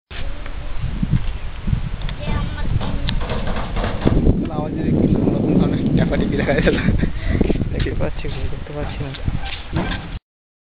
child are playing
this is very nice river and very old temple.
you will enjoy the video.